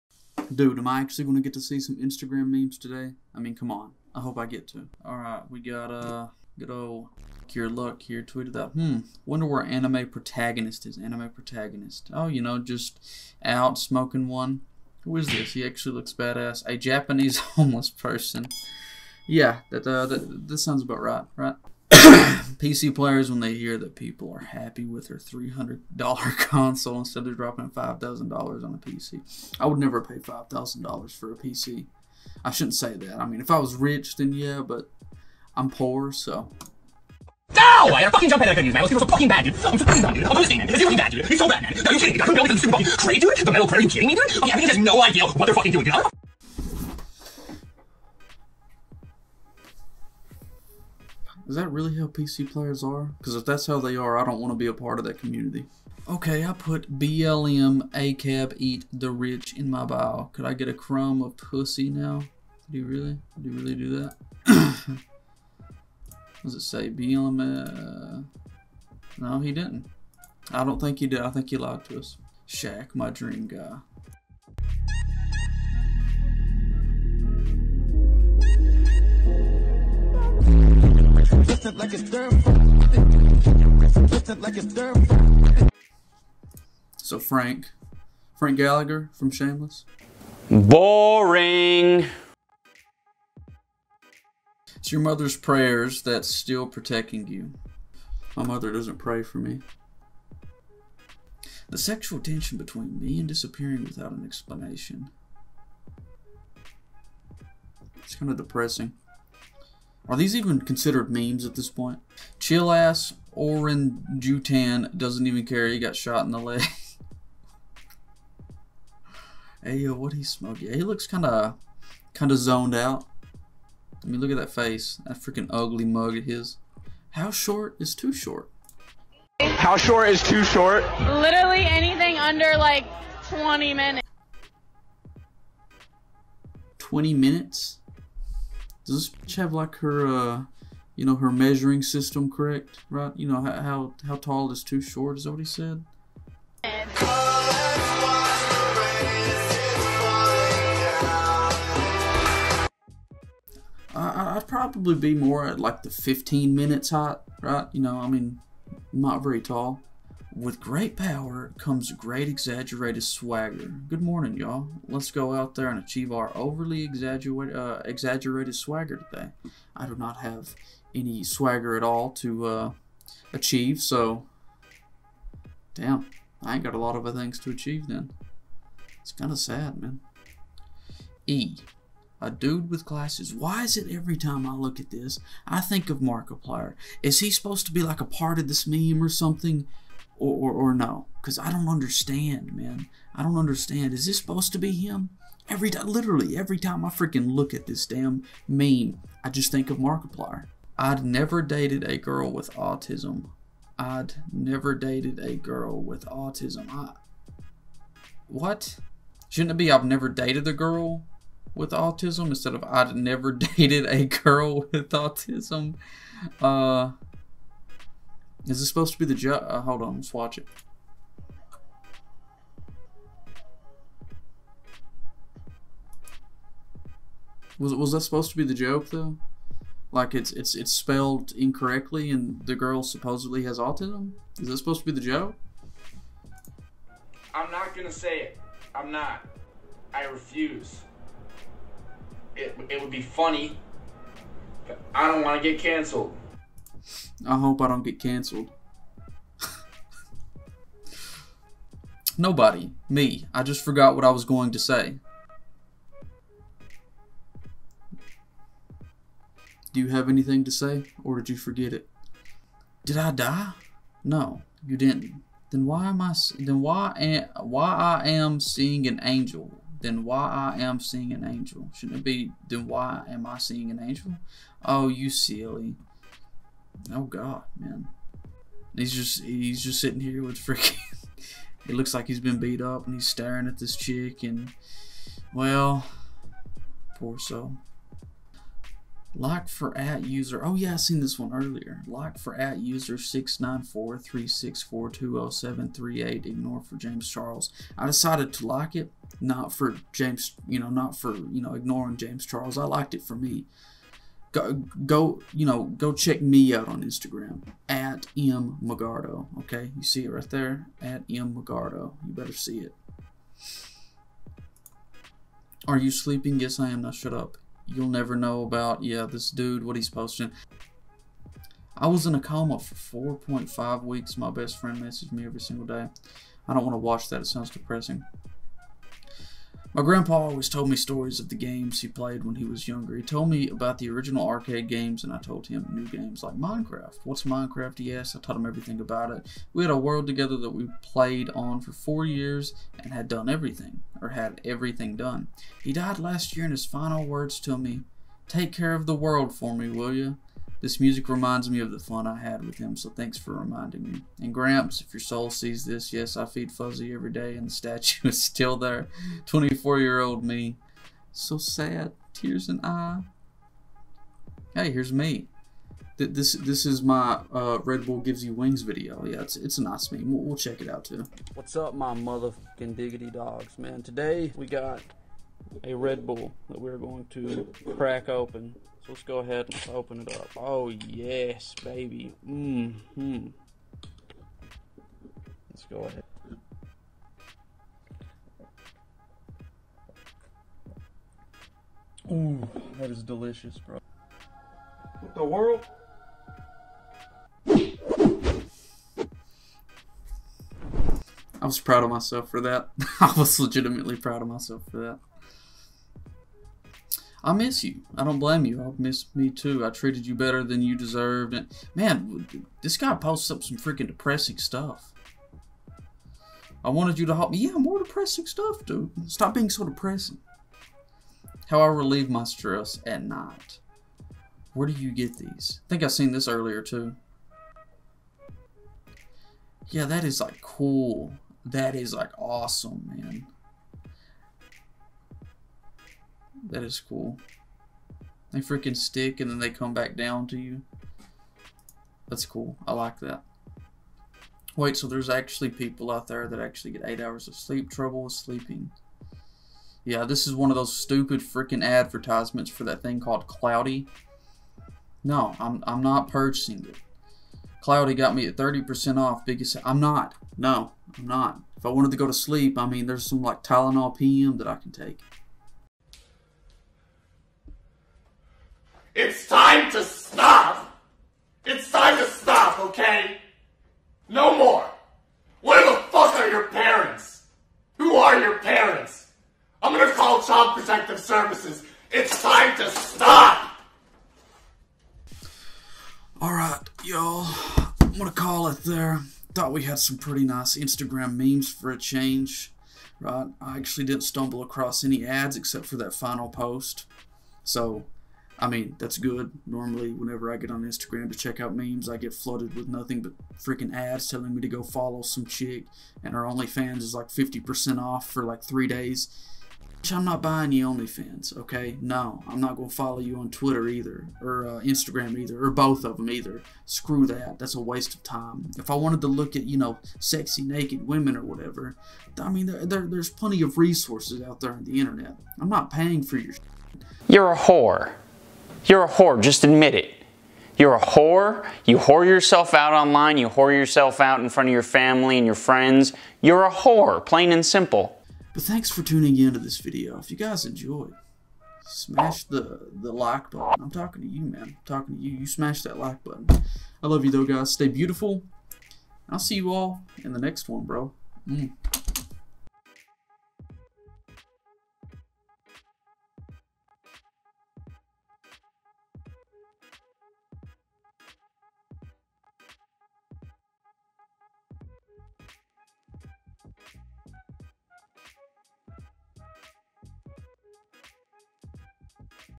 Reacting to Boring Instagram Memes! YOU WILL NOT LAUGH!
I had nothing to do with my time, so I looked at some Instagram memes. But for once, I wasn't really flooded with ads, so it was a good day!

Warning: A lot of the memes were kinda boring, so you probably won't laugh.